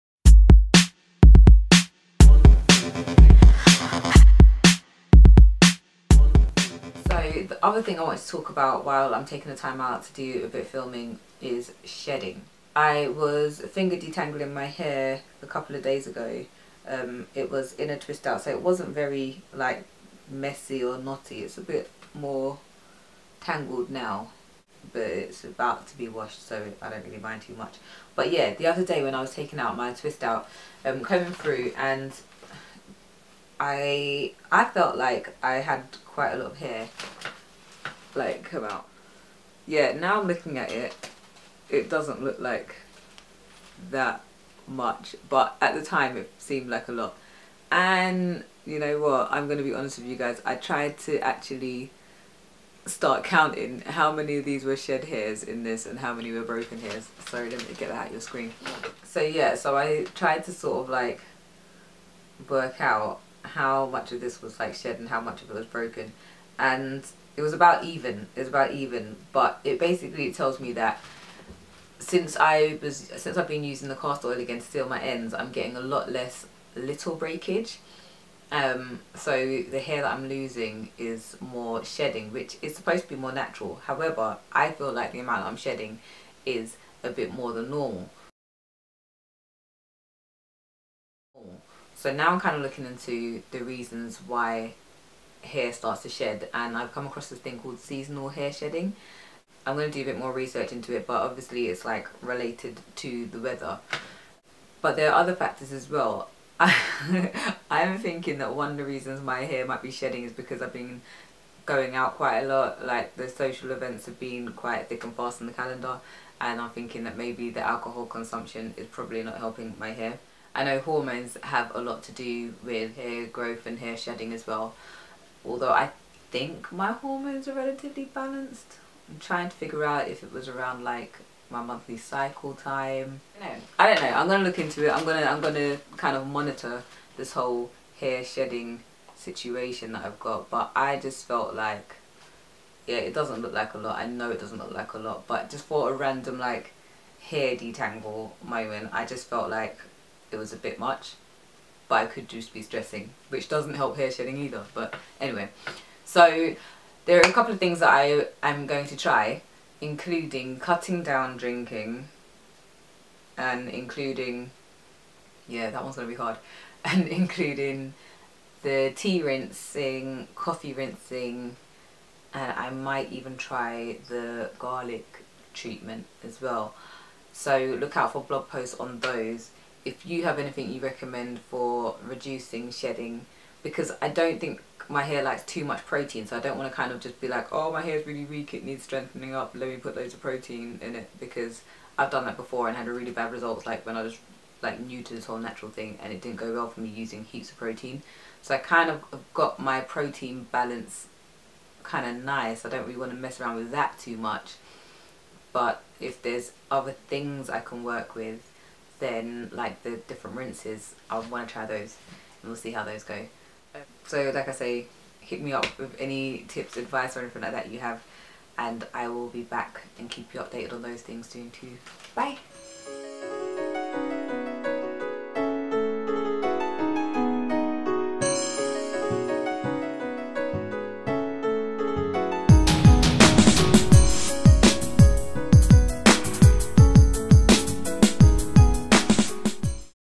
Natural Hair & Shedding
After detangling my hair a few weeks ago, I noticed that more hair than usual came out. I have a lot less breakage since I've started using jamaican black castor oil, again, and after checking I realised that quite a lot of the hairs were shed hairs.

After filming this video I did some research on the causes of shedding.
Check Out the blog post: 5 Causes Of Natural Hair Shedding